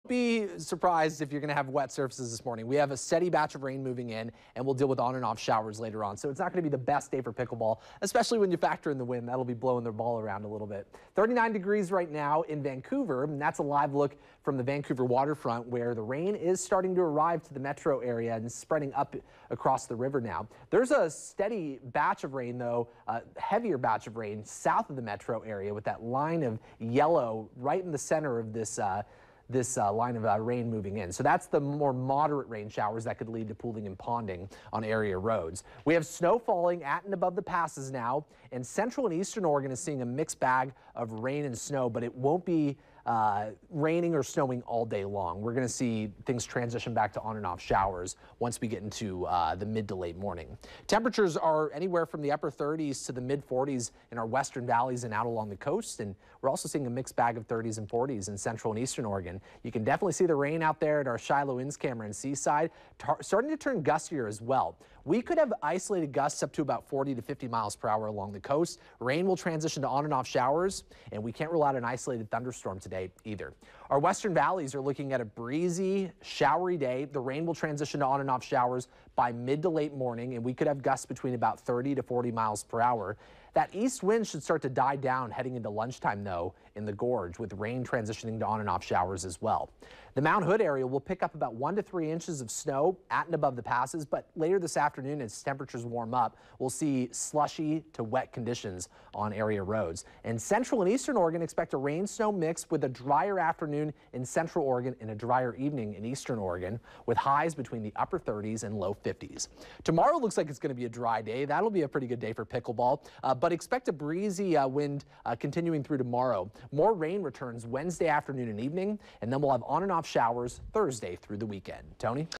First Alert Monday morning FOX 12 weather forecast (1/9)
Here is the weather forecast for the morning of Monday, January 9, 2023.